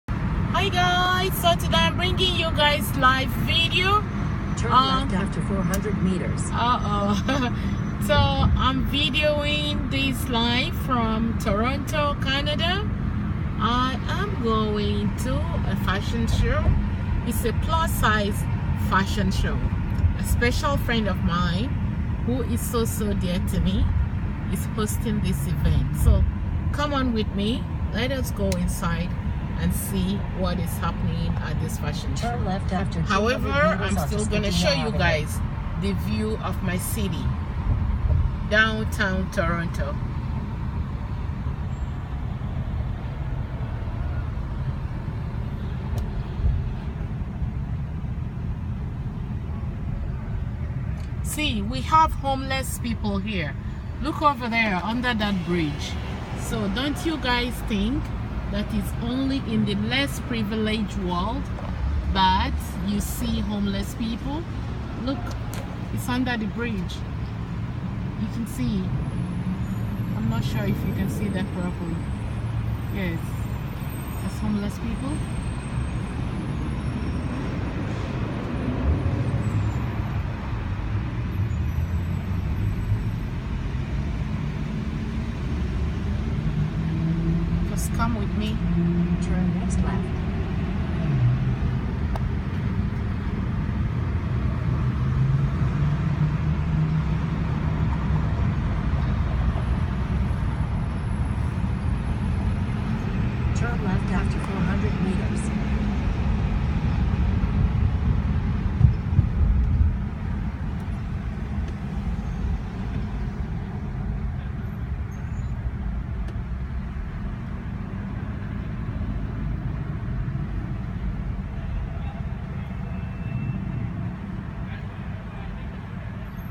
Welcome to Toronto Downtown , Homeless under the bridge :Vlog Episode
Driving to fashion show, view of downtown Toronto: Vlog 16